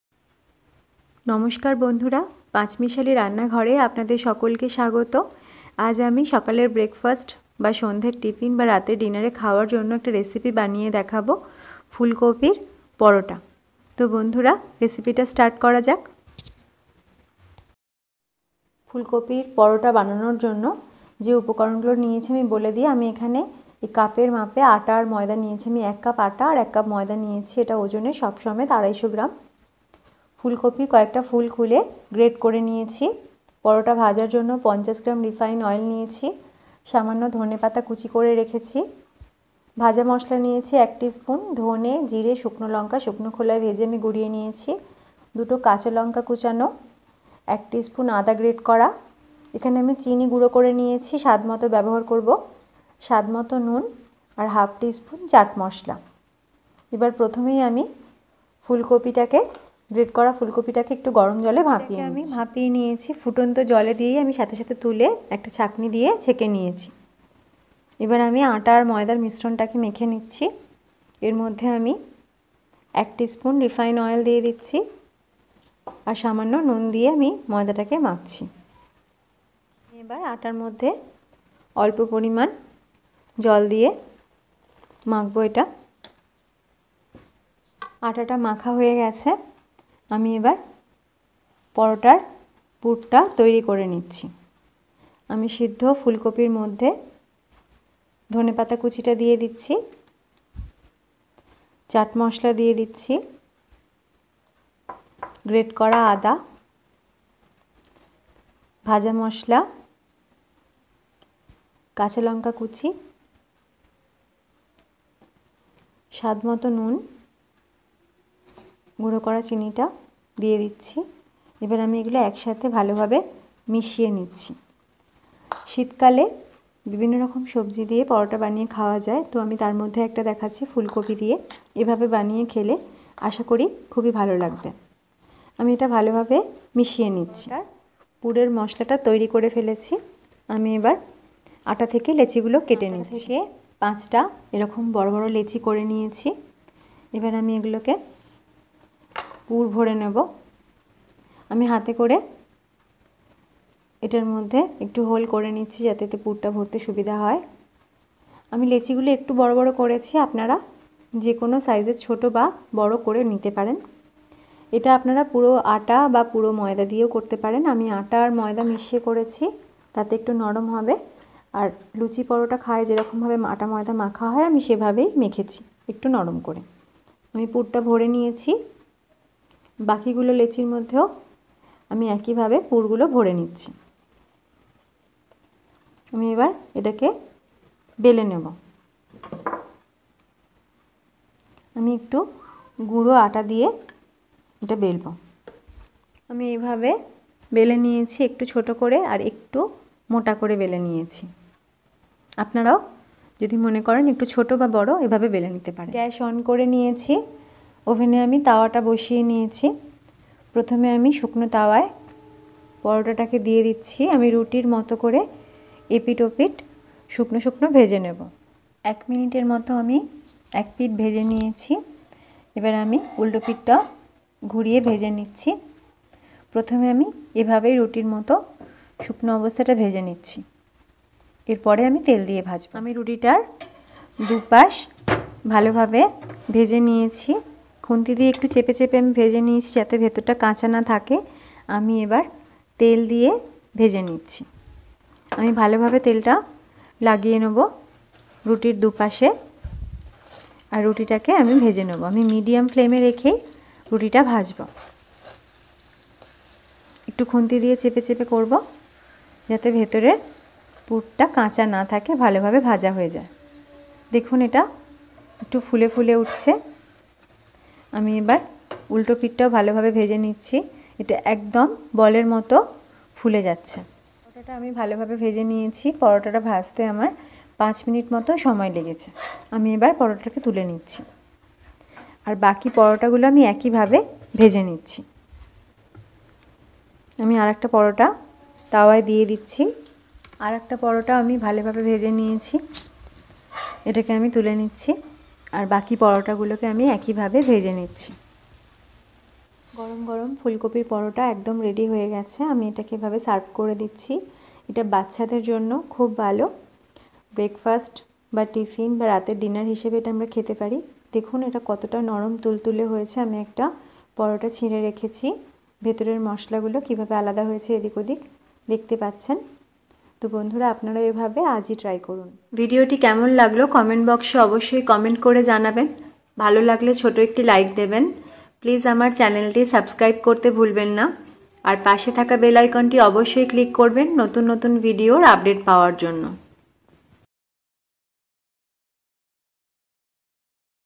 ফুলকপির পরোটা রেসিপি।একদম সহজ পদ্ধতিতে ফুলকপি দিয়ে পুর ভরা পরোটা রেসিপি।Fulkopir Porota Recipe
আজ আমি আপনাদেরকে খুব সহজ পদ্ধতিতে ফুলকপির পরোটা বানিয়ে দেখাবো। এটি শীতের সকালে জলখাবারে খাবার মত একটি দুর্দান্ত রেসিপি।

উপকরণ:
ময়দা
ফুলকপি গ্রেট করা
ধনেপাতা কুচি
কাঁচা লঙ্কা কুচি
আদা গ্রেট করা
ভাজা মশলা (ধনে জীরে শুকনো লঙ্কা শুকনো খোলায় ভেজে গুঁড়িয়ে রাখা)
চাট মশলা
নুন
সাদা তেল
গুঁড়ো করা চিনি

ভিডিওটি কেমন লাগলো তা কমেন্ট বক্সে কমেন্ট করে জানাবেন।
ভিডিওটি ভালো লাগলে ছোট্ট একটি লাইক করবেন।